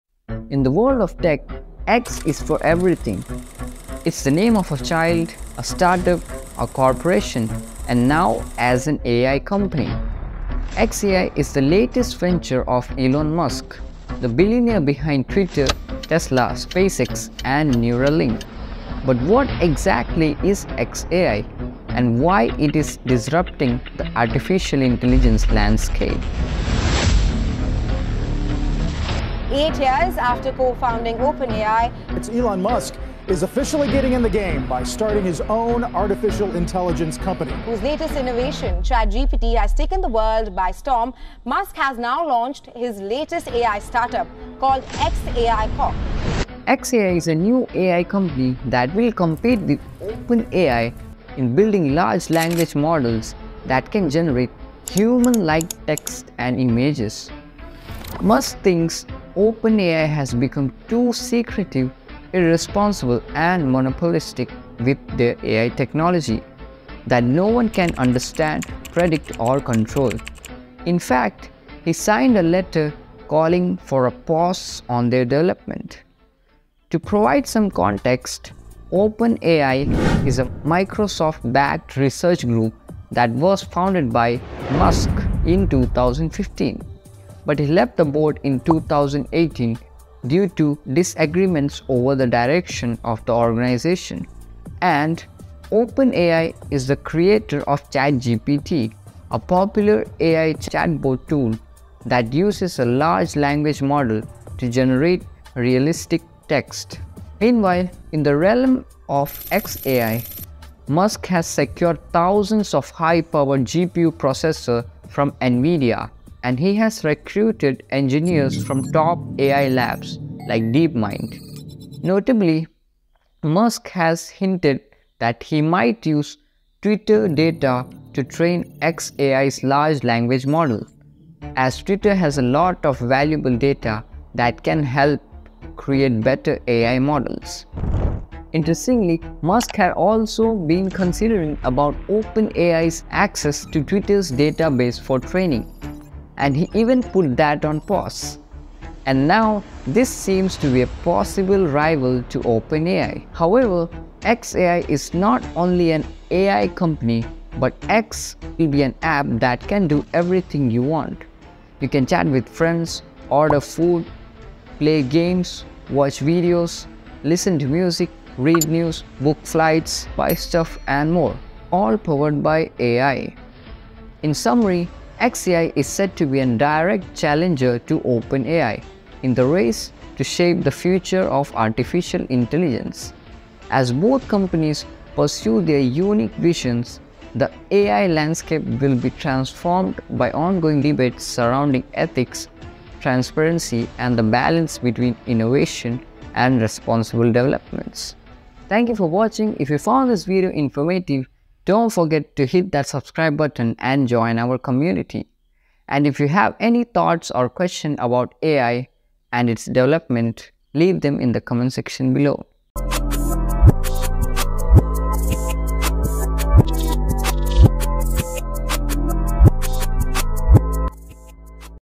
Elon Musk's Secret AI Project Challenging OpenAI
X.AI is a new company that Musk founded in March 2023, with the goal of creating an app called Everything X, which will be a one-stop shop for all your needs12.

X.AI is also developing a powerful generative AI model that will compete with ChatGPT and GPT-4, the state-of-the-art language models created by OpenAI23. X.AI has hired some of the best AI engineers from Google's Deepmind and OpenAI, and has reportedly purchased thousands of GPUs to power its AI product245.

Why did Musk start X.AI? What is his vision for Everything X?

Listen to My Songs-